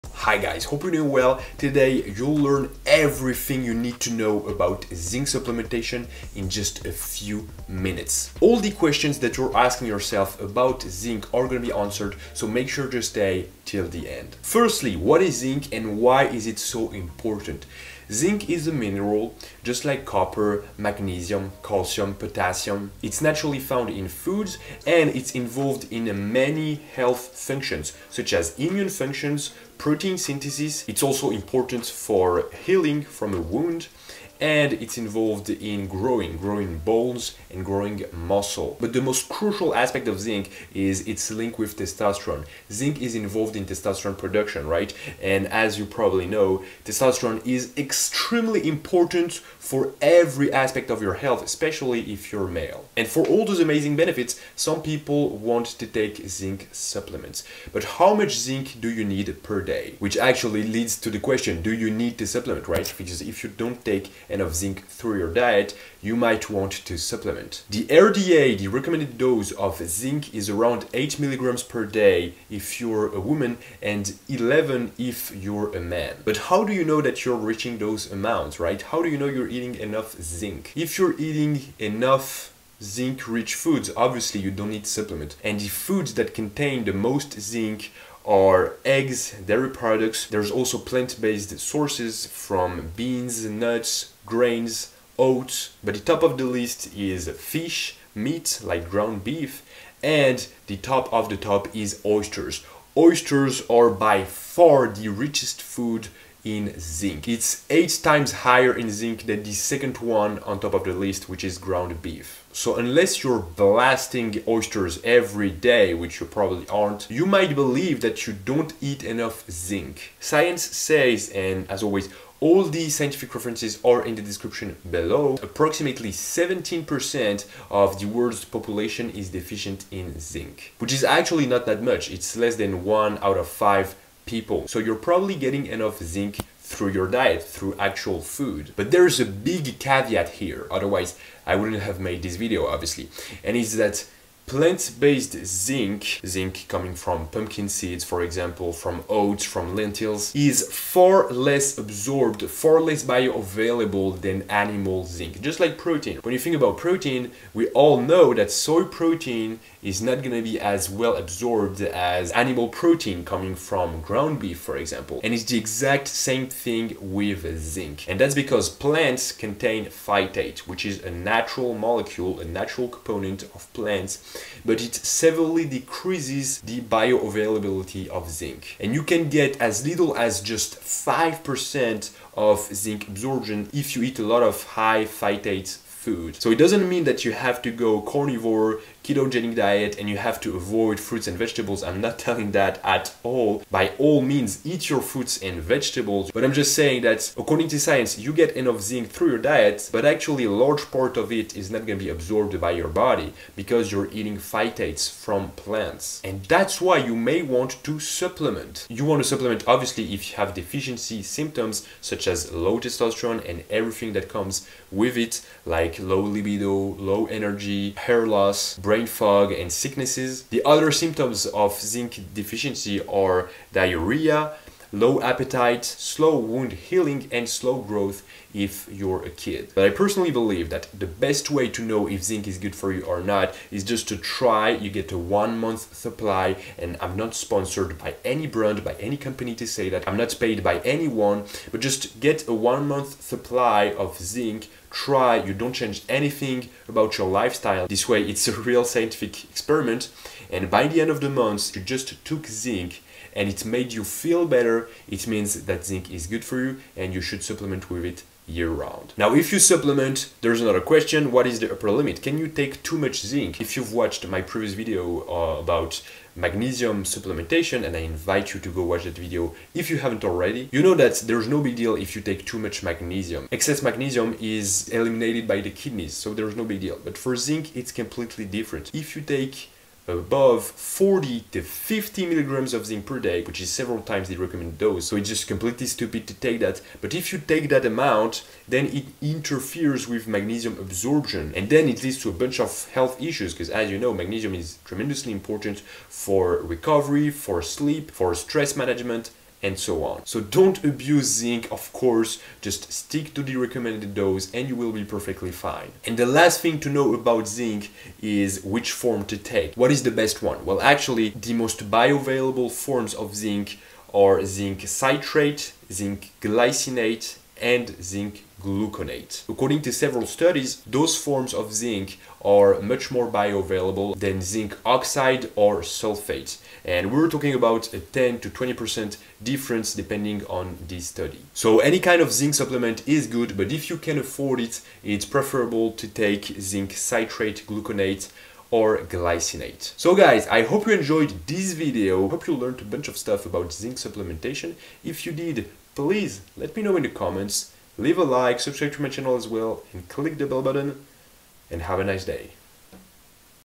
EVERYTHING you need to know about ZINC (in 6 min)
What is zinc, and do you truly need to supplement ? Let's answer those questions and explain everything about zinc supplementation in no time.

Timestamps:

0:00 Intro
0:14 What is zinc?
0:53 How much zinc do you need?
1:16 How to know if you're getting enough zinc?
2:18 The problem of zinc in plant-based foods
3:29 Do you need to supplement?
4:19 Is there an upper limit?
5:06 What type of zinc is better?
5:40 Recap & outro